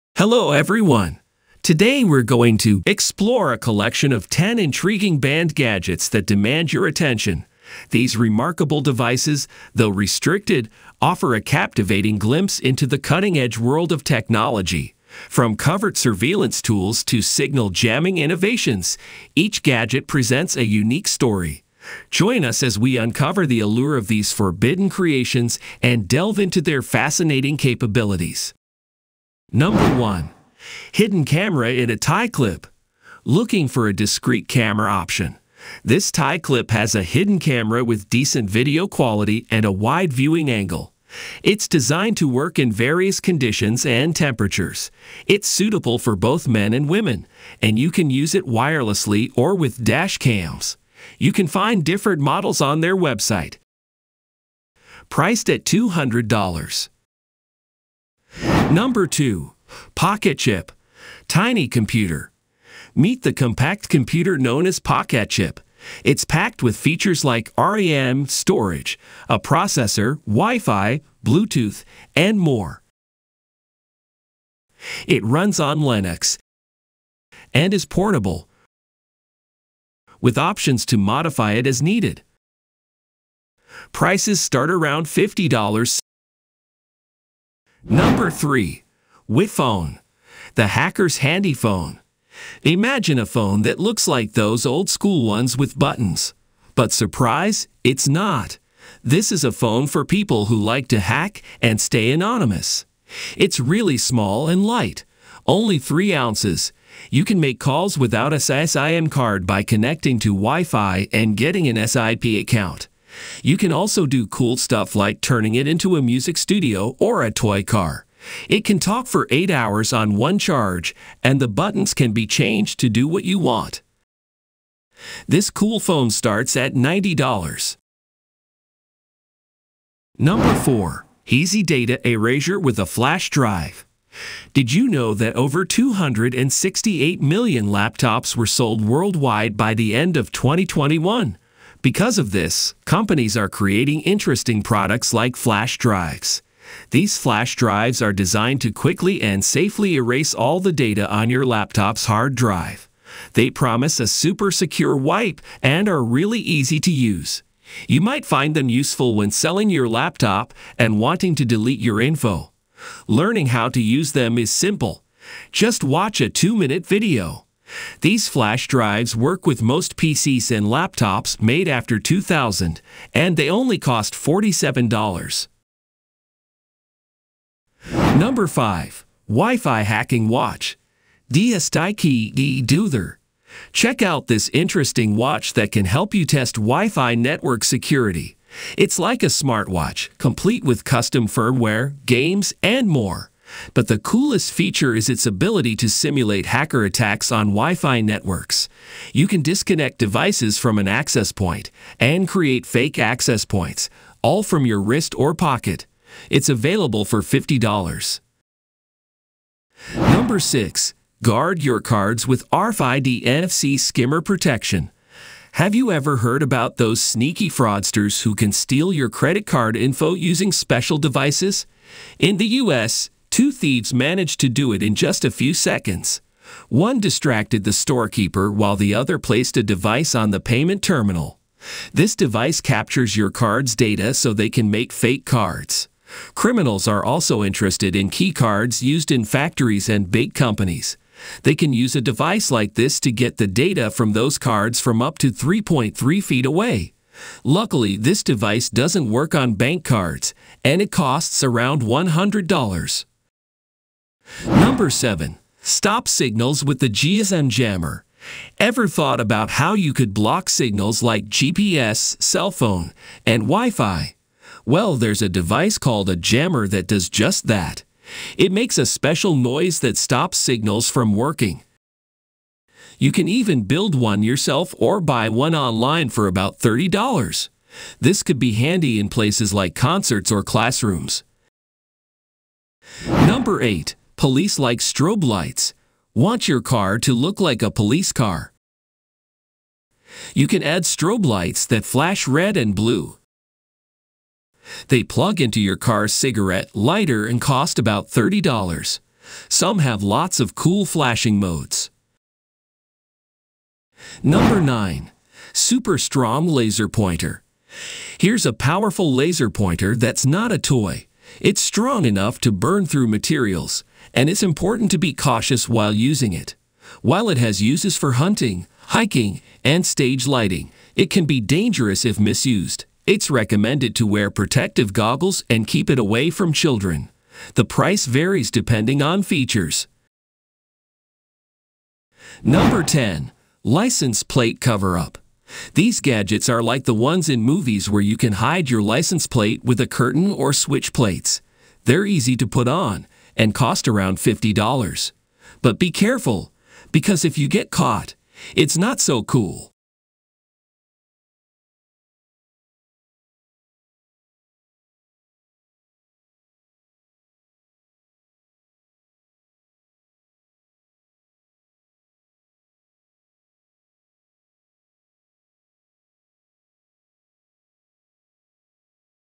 10 BANNED GADGETS YOU MUST SEE in 2024
Dive into the future of technology with '10 BANNED GADGETS YOU MUST SEE in 2024.' Uncover the latest innovations that have stirred controversy and faced bans. From groundbreaking advancements to contentious breakthroughs, this video explores the cutting edge of gadgetry. Witness the clash between innovation and restriction as we showcase gadgets that redefine possibilities in 2024. Join us on a journey through the forbidden realm of tech, where creativity meets resistance, and the future takes center stage.